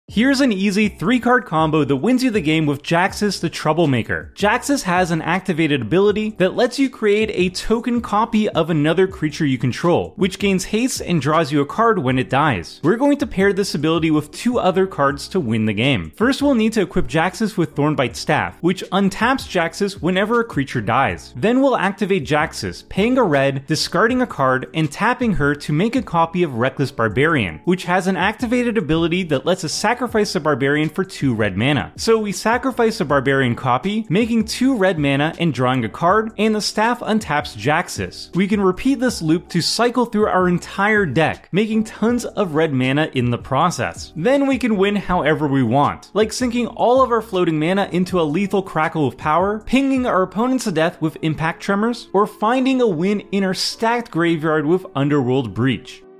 Draw Deck & Make Tons Of Mana With Jaxis! | Commander MTG
Jaxis has some awesome combos to close out games!